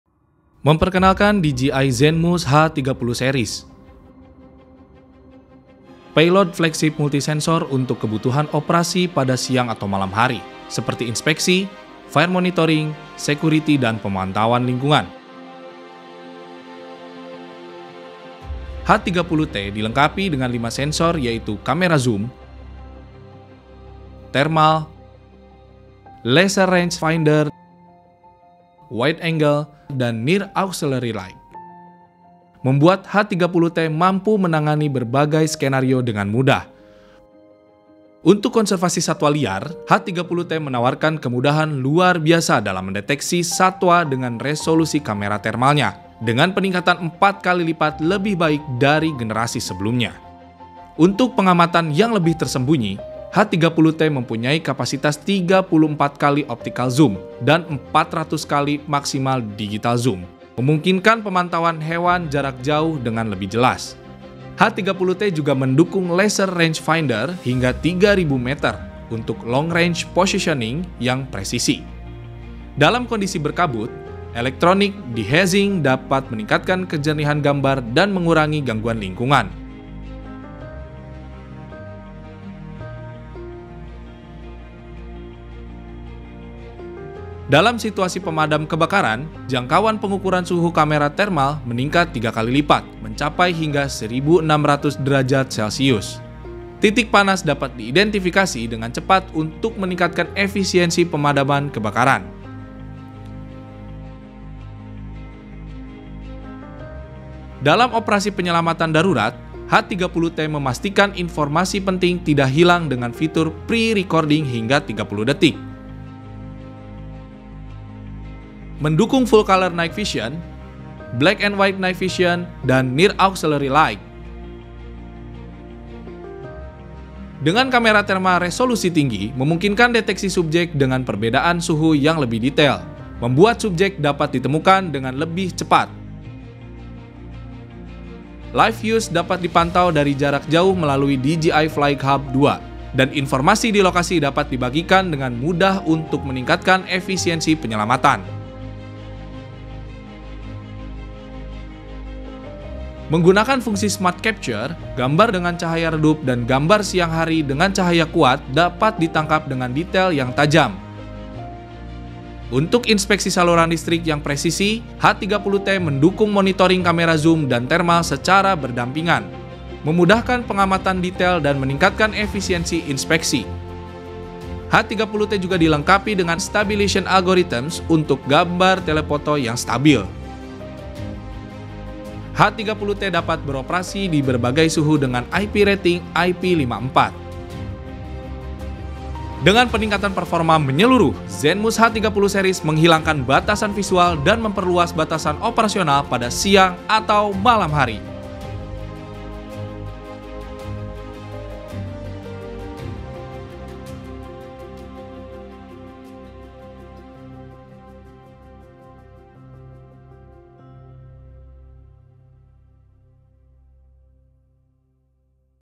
DJI Zenmuse H30 Series: Solusi Payload Multi-Sensor untuk Operasi Siang dan Malam
DJI kembali menghadirkan terobosan dengan meluncurkan DJI Zenmuse H30 Series, payload multi-sensor yang terdiri dari Zenmuse H30 & H30T. Dengan menggabungkan full-color night vision dan kamera thermal, payload DJI terbaru ini menawarkan solusi ideal untuk berbagai kebutuhan operasi siang dan malam, seperti fire monitoring, inspeksi, security, hingga kebutuhan penelitian lingkungan.

DJI Zenmuse H30T dilengkapi dengan 5 sensor, kamera thermal beresolusi tinggi 1280x1024, kamera zoom 40MP dengan kemampuan 34x optical zoom, kamera wide-angle 48MP, laser range finder dengan jangkauan 3-3000m yang presisi, dan NIR auxiliary light (illumination range: 100m).